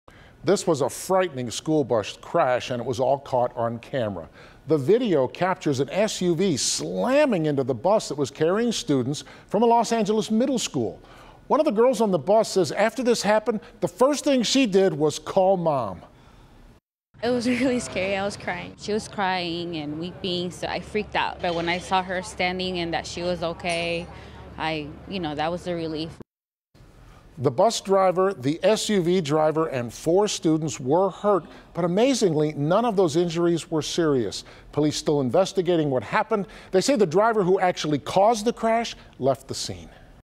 Video shows school bus crash in Los Angeles
A dangerous school bus crash in Los Angeles was caught on camera.